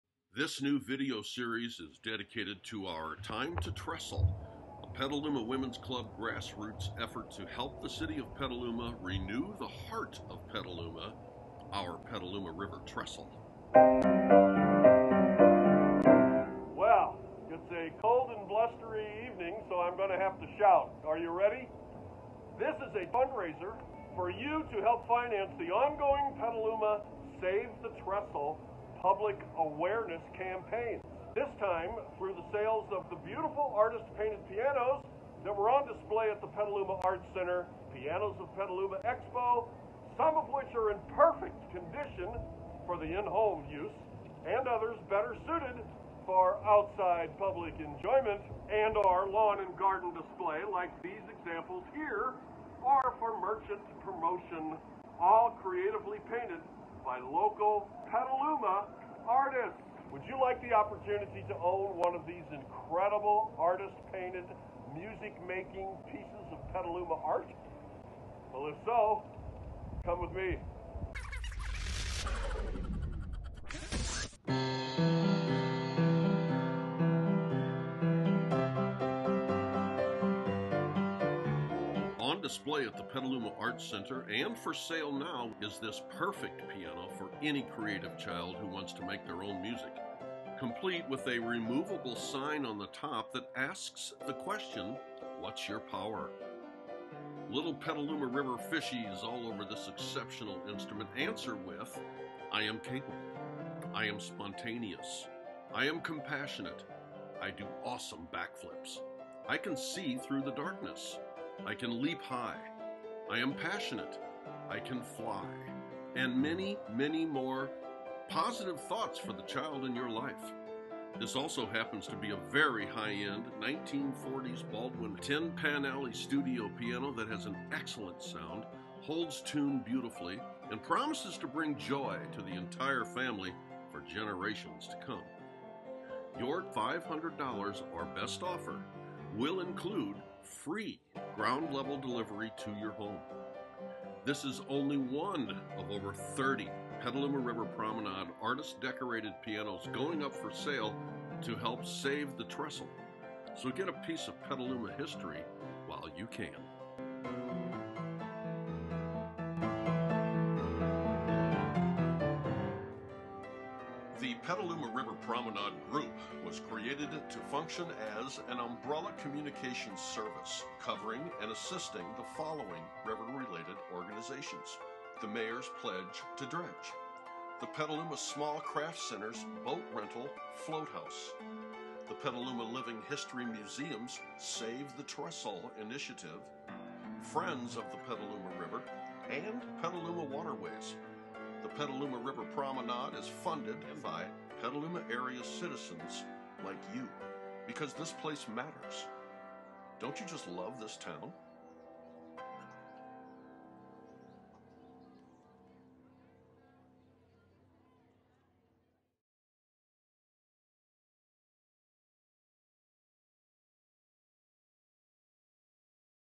Buy any one of these Petaluma local artist-painted pianos!
Petaluma Pete see it's "Time to Trestle" with your purchase of any one of these Petaluma River Promenade local artist-painted pianos!